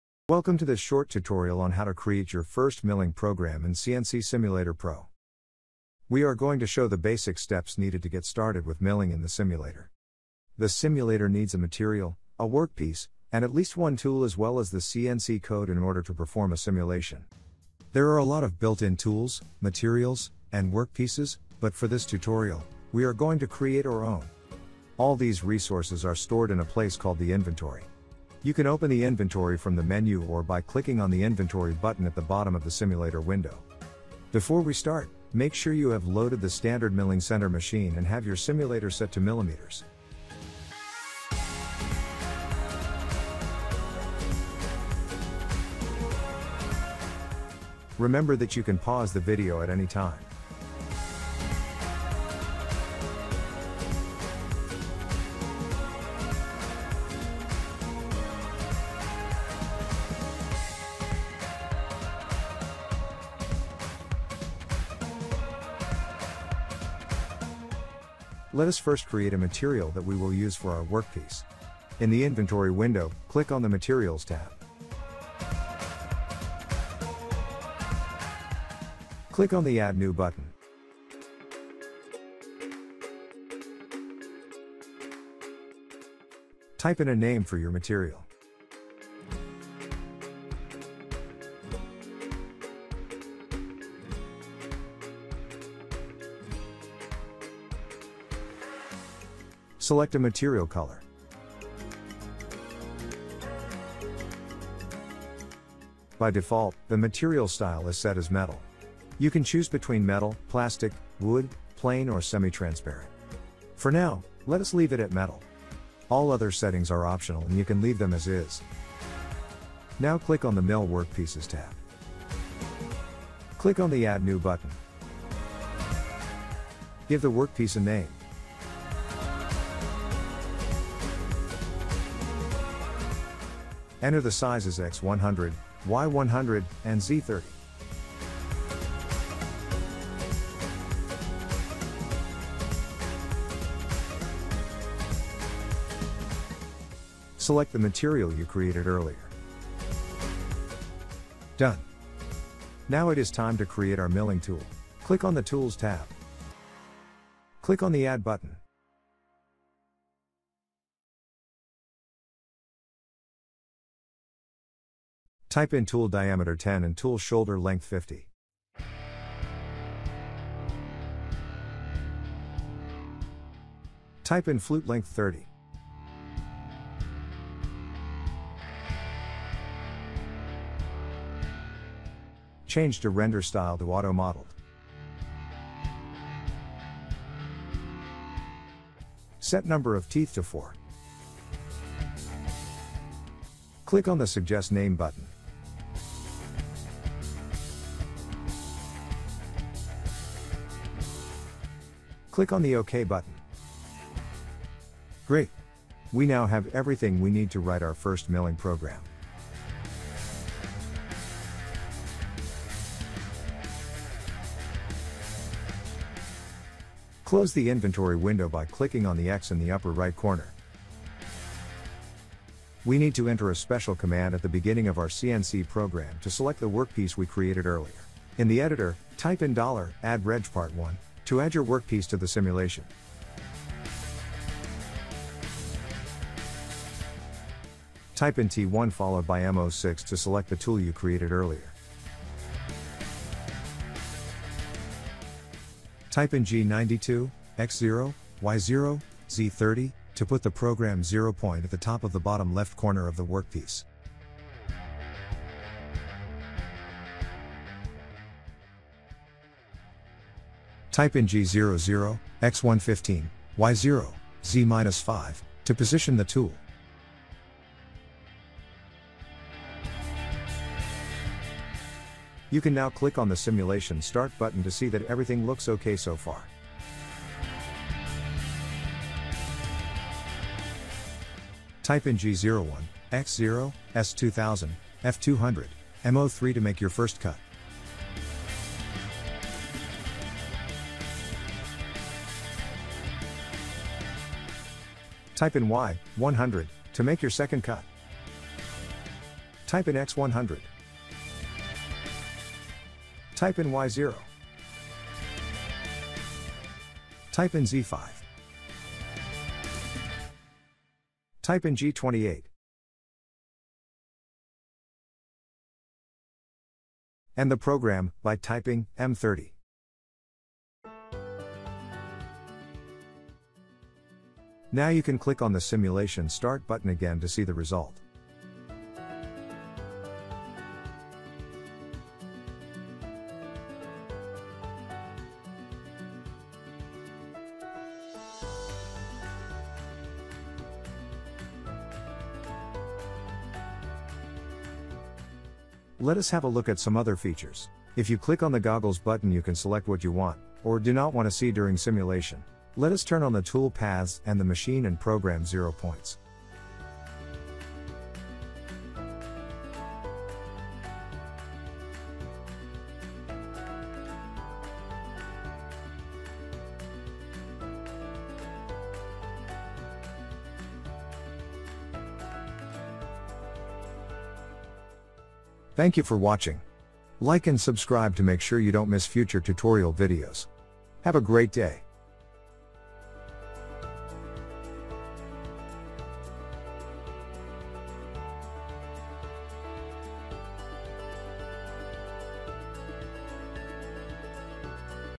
How to create your first milling program in CNC Simulator Pro
A quick tutorial on how to get started with milling in the simulator version 4  (soon to be released).